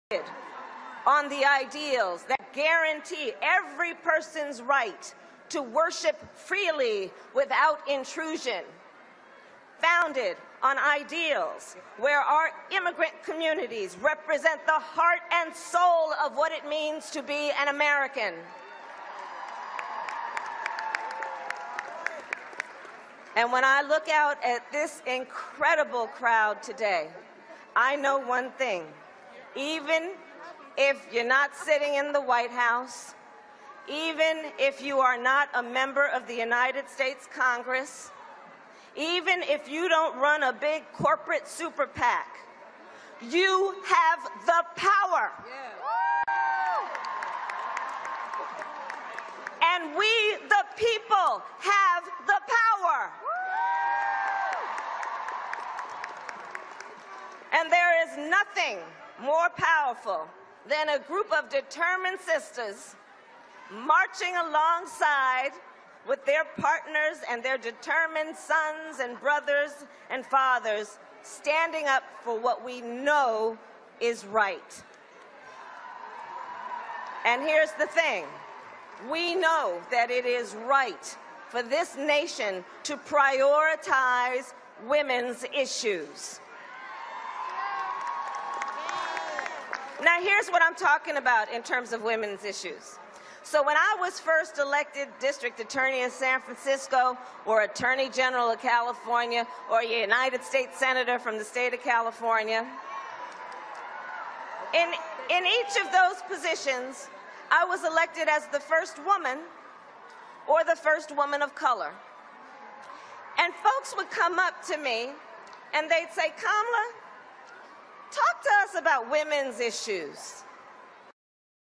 Women's March: Sen. Kamala Harris speaks at the "We, the people, have the power"
Watch Sen. Kamala Harris speaks at the WomensMarch: "We, the people, have the power".